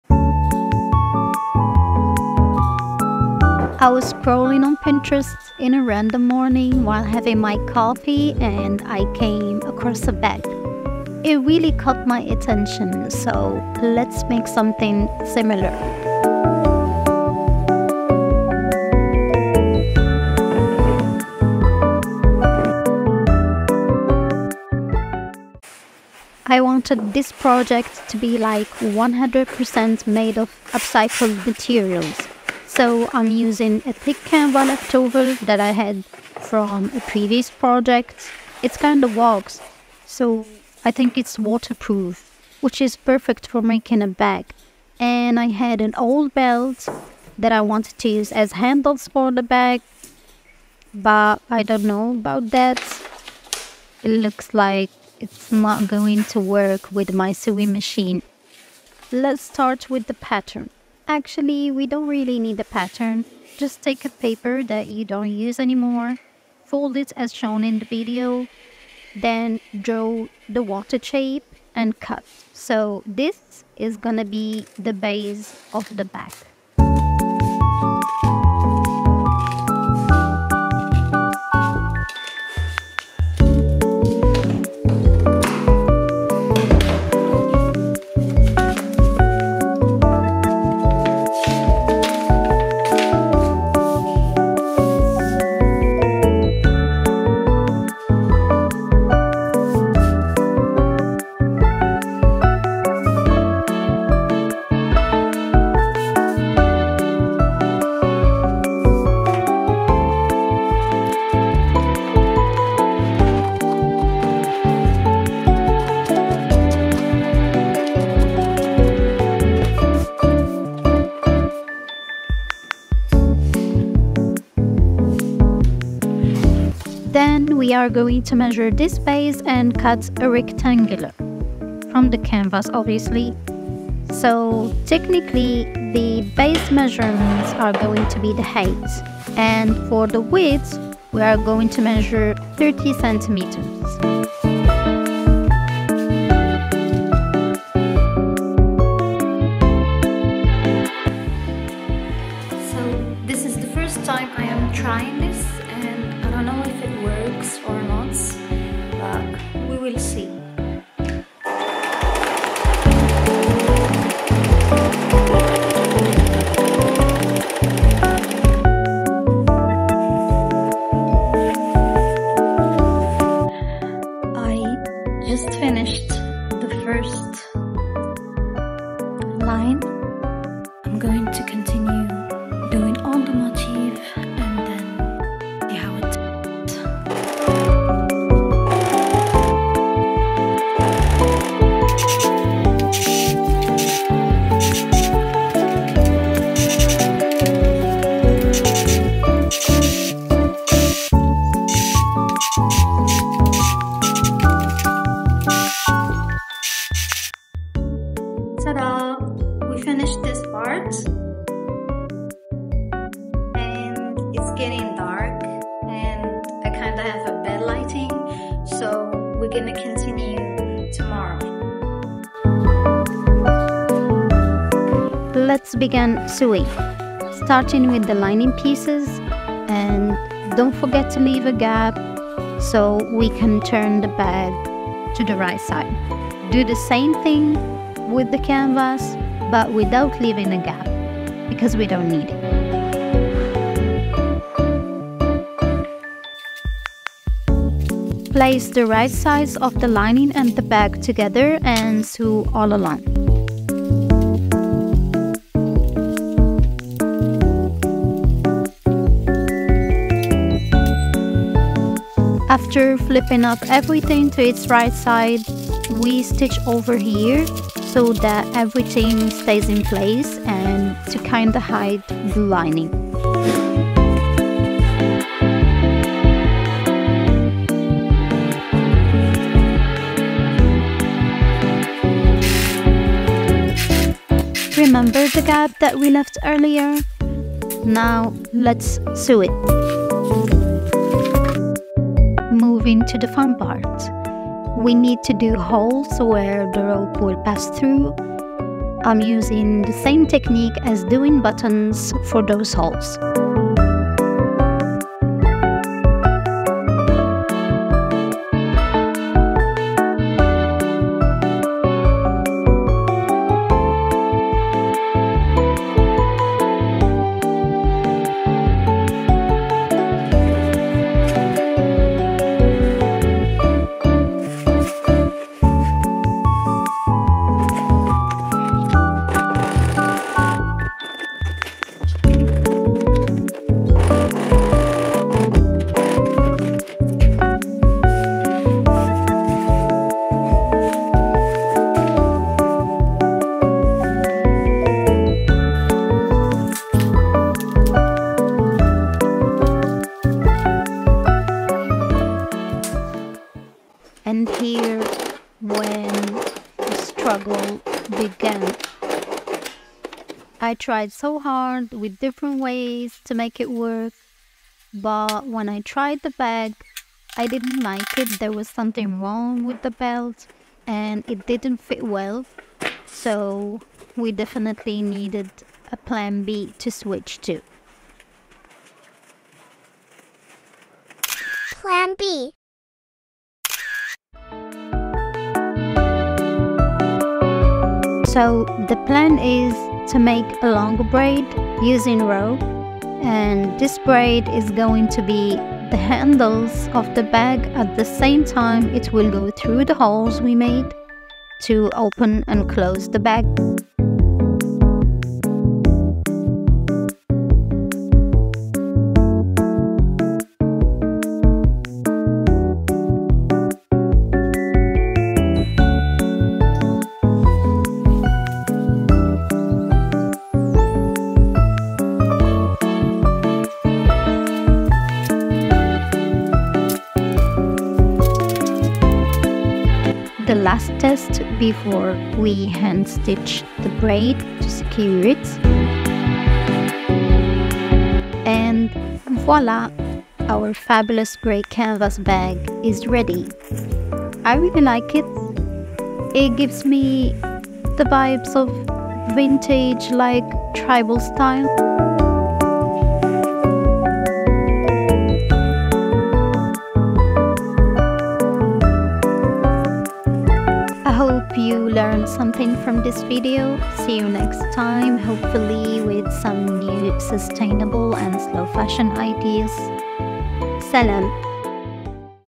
Sewing Tutorial : Making a bag using upcycled materials
salam alaikum/hello guys, in this video I collected all the leftovers and non-used materials cooked them all to come up with something very interesting.This bag was created with a vision of "using trash to make art" and the result was captivating a bag that has vintage authentic vibes, if you like this kinda tutorials please let me know in the comments let's build a community over here and discuss things about slow Fashion and zero waste Sewing...etc